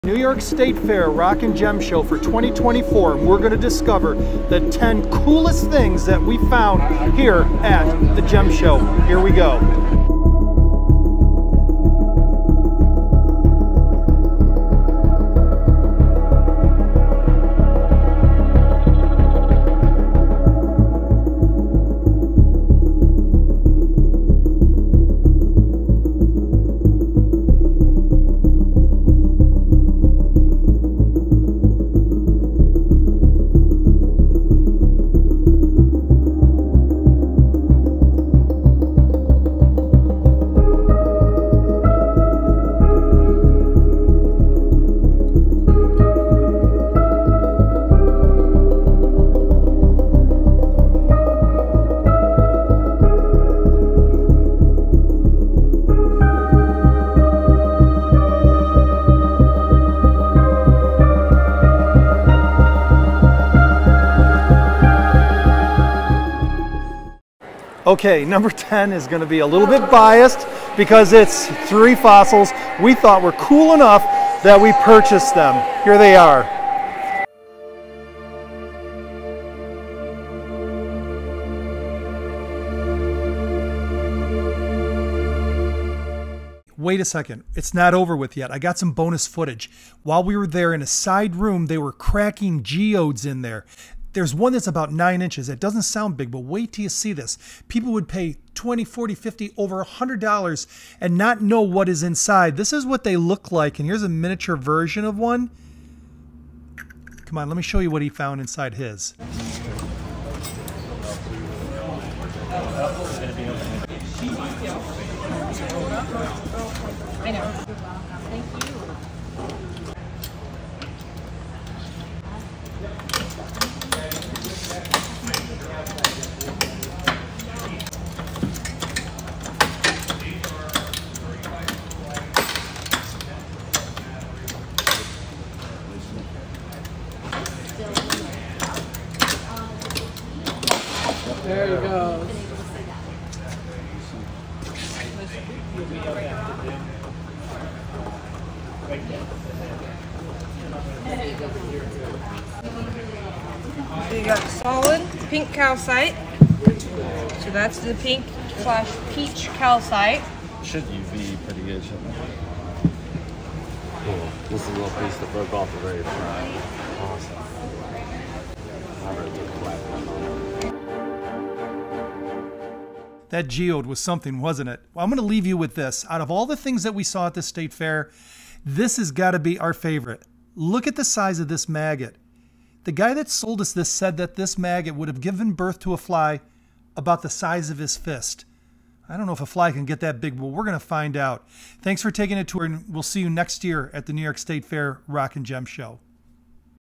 Our Top 10 Picks - Fossil & Gem Show 2024
We picked the top 10 rocks, fossils and or gem stones while walking the aisles at the New York State Fairgrounds. We picked the top 10 that were the coolest and most impressive at the 2024 Rock & Gem show. And we purchased #10. Plus at the end  of this video we have a bonus geode cracking session.